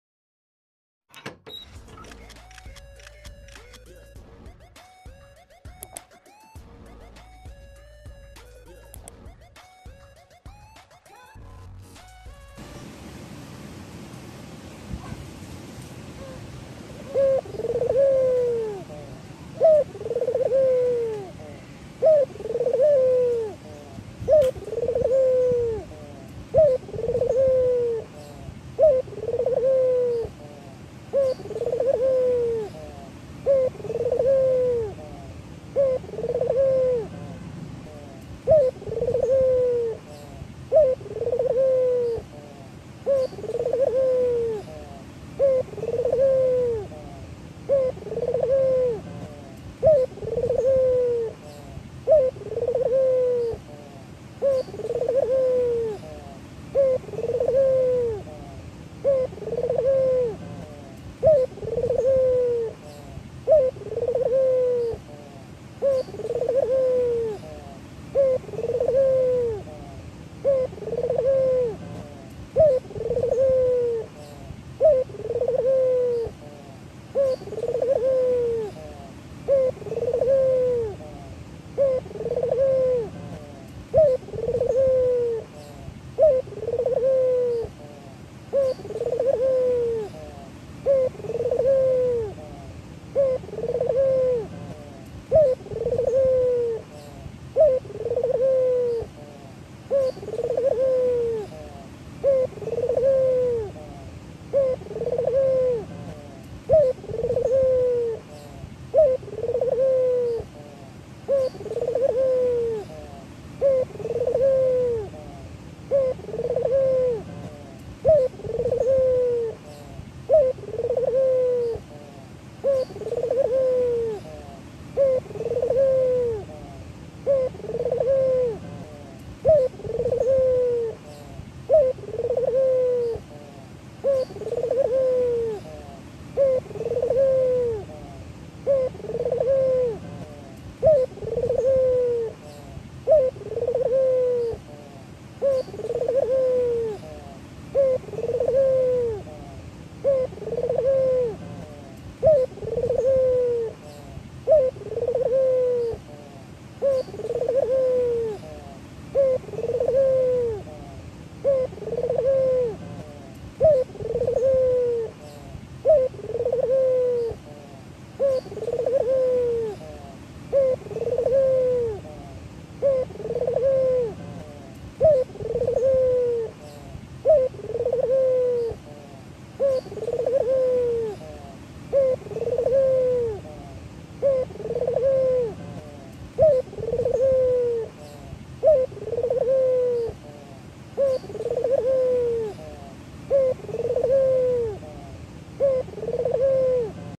Suara Burung Puter Gacor , merangsang burung puter agar bunyi | burung puter Lokal jantan gacor .
Assalamualaikum Wr.Wb.
Dan silakan beri saran dikolom Komentar !

Silakan kunjungi juga Playlistnya